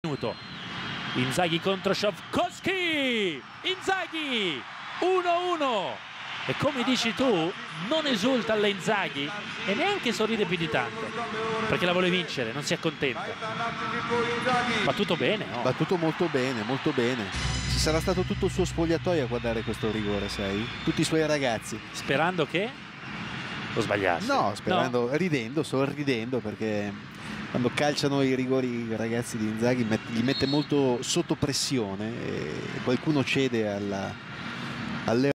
Inzaghi Goal vs Kaladze Team 31-05-2013
AC Milan vs Kaladze Team - addio kaladze - goodbye Kaladze